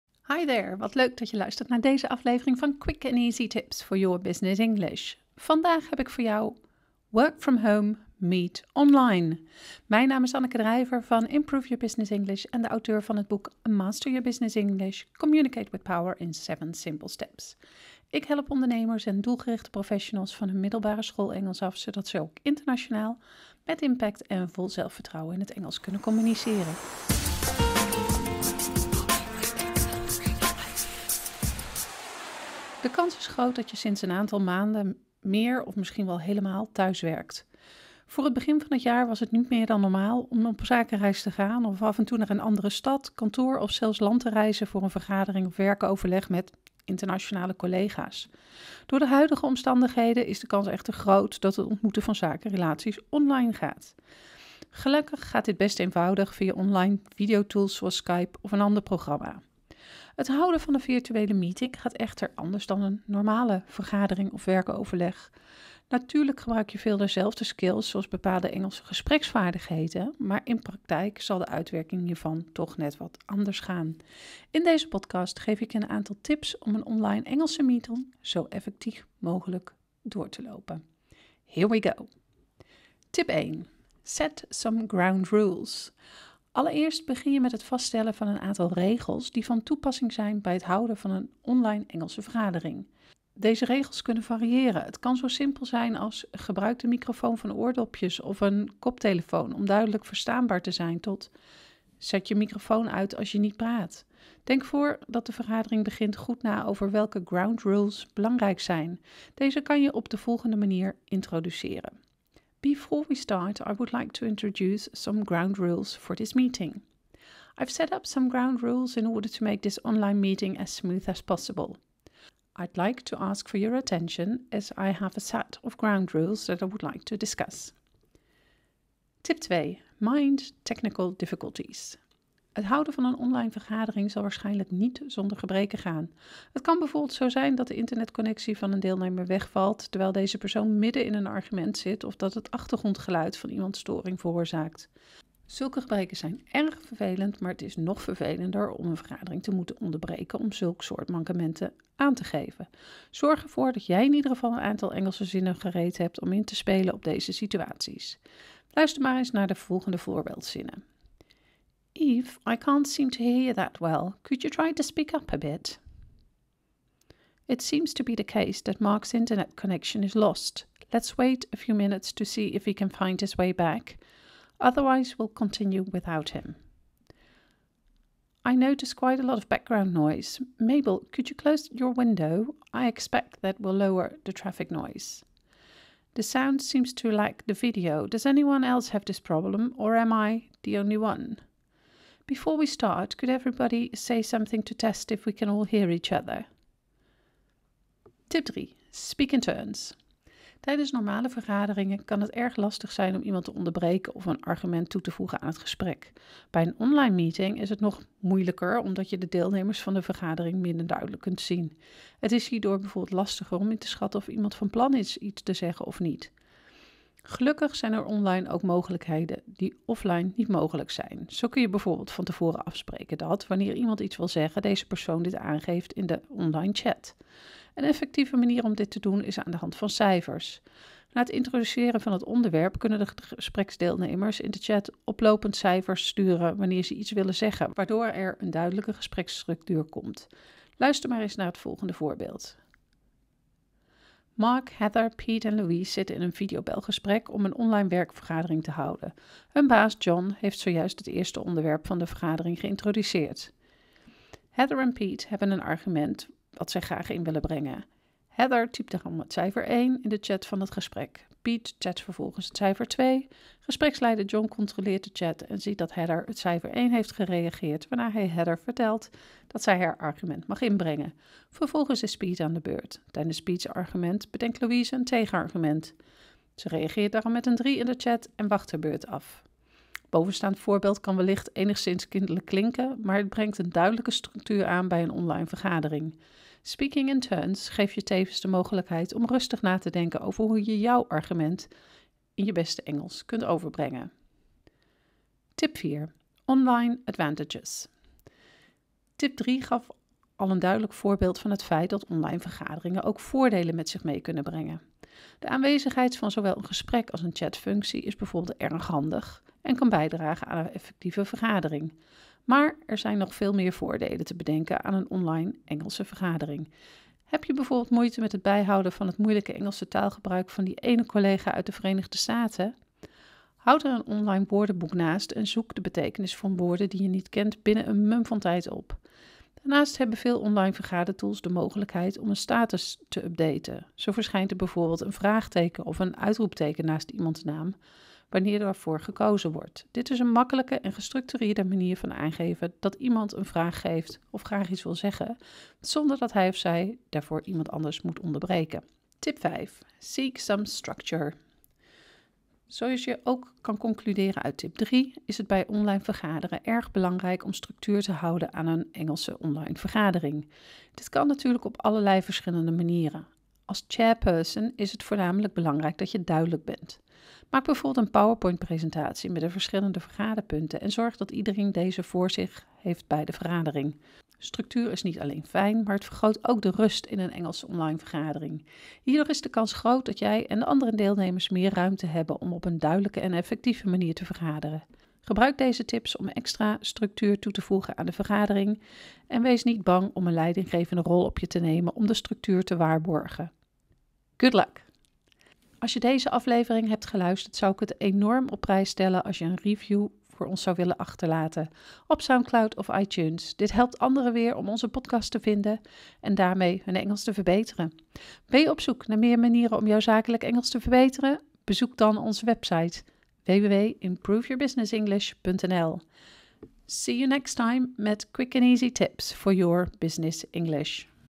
#082 Work from home, meet online - Quick & Easy Tips for your Business English
Online Engels vergaderen? Roept dit spanning of onzekerheid bij je op? Niet gevreesd! Ondanks dat een online Engelse meeting net wat anders verloopt dan een offline meeting hoeft het niet te betekenen dat jij niet professioneel kan overkomen. Deze podcast geeft je namelijk tips en advies over hoe jij je hoofd koel houdt bij een Engelse vergadering in een online omgeving. Work from Home, Meet Online!

Luister je de podcast liever via onze website? Ga dan naar

Benieuwd naar meer quick and easy tips om jouw zakelijk Engels te verbeteren?